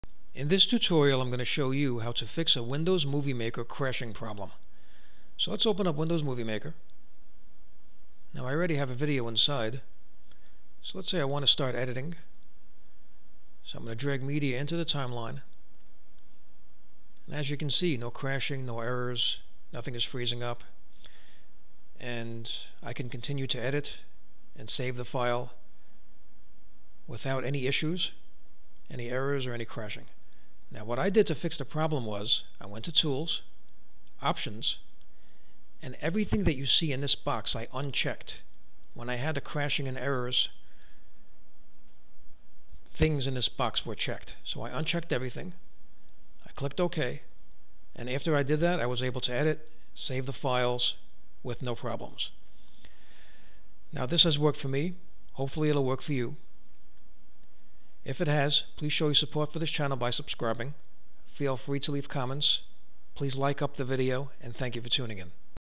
How To Fix A Windows Movie Maker Crashing Problem
This tutorial will show you how to fix windows movie maker from crashing, freezing and getting an error message